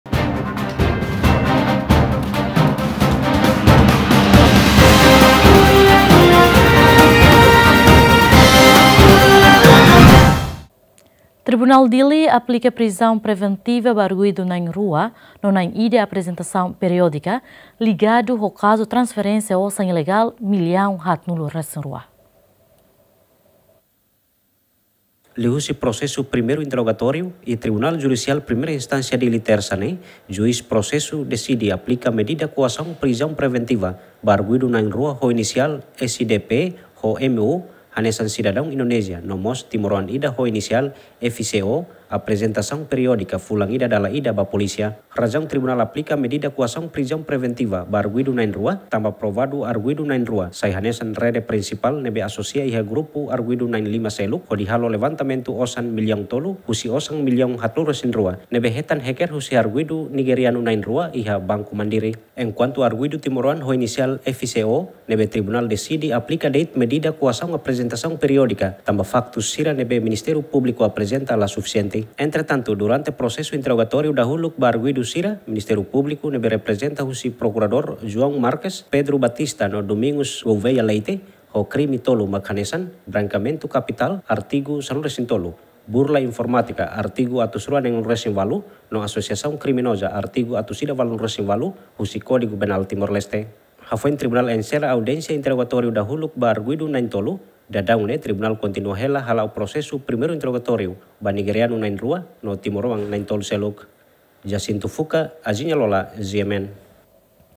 TD aplika prizaun preventiva ba arguidu na'in 2 no nain 1 aprezentsaun periodika ligadu ho kazu ...
Tribunál Dili aplika Prizaun Preventiva ba arguidu na'in 2 no nain 1 aprezentsaun periodika ligadu ho kazu transferénsia osan ilegál millaun 42